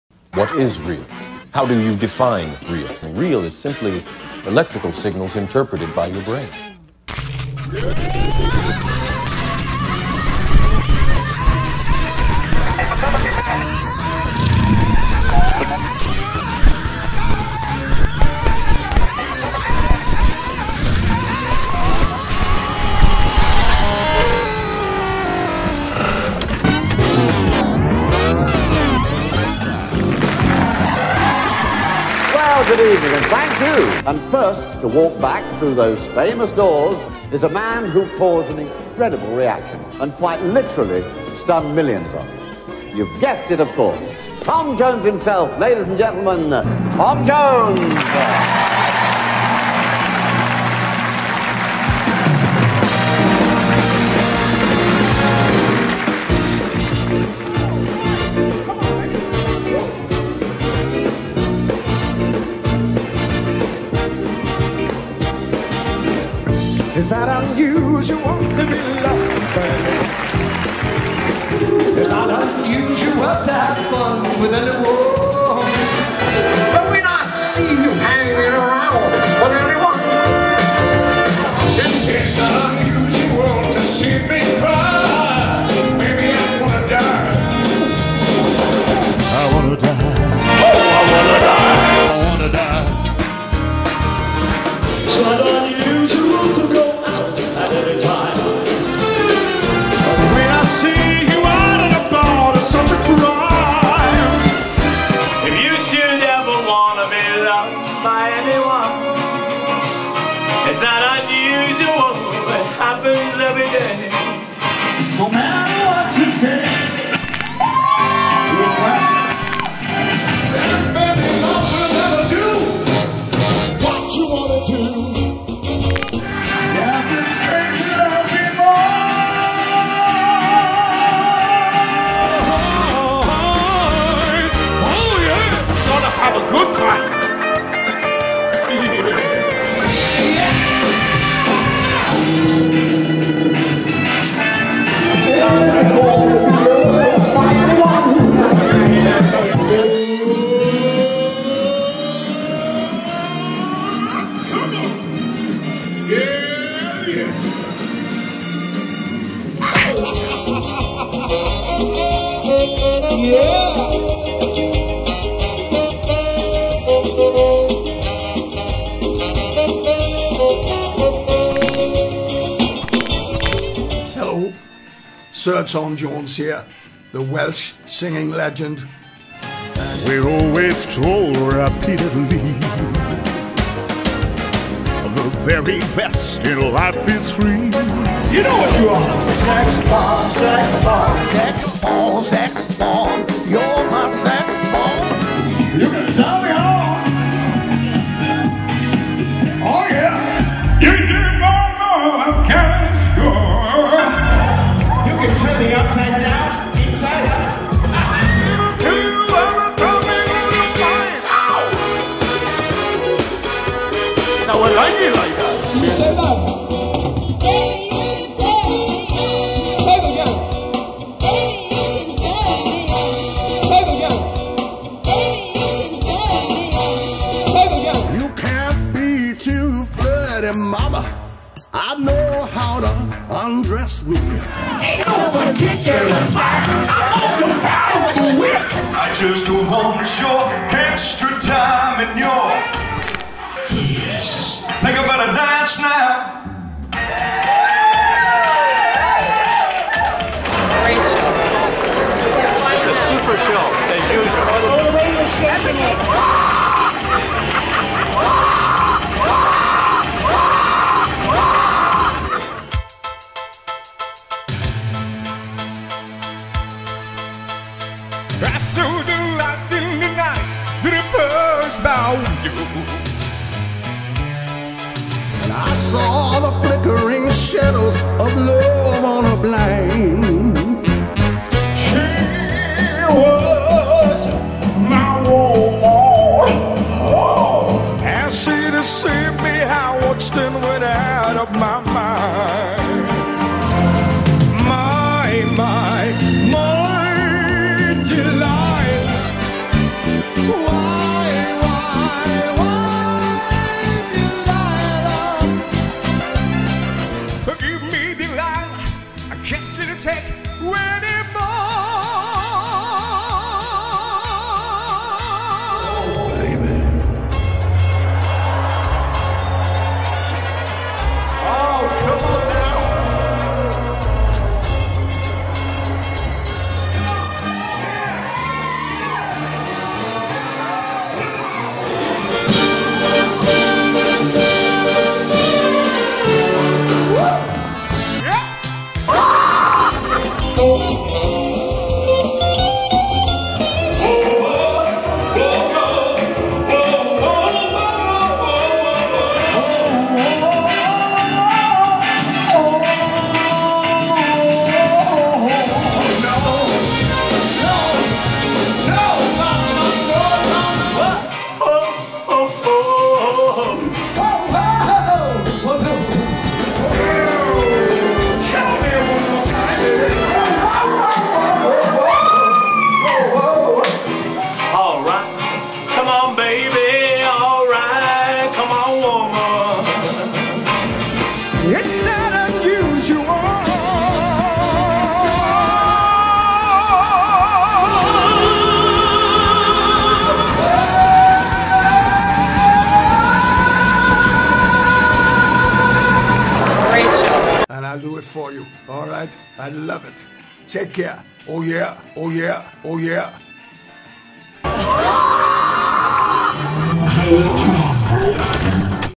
Tom Jones Isn't Real, and it's Unusual
For centuries, people have questioned if Tom Jones is real. The answer is that there are so many of him in the world there is no one entity that is Welsh singer, Tom Jones.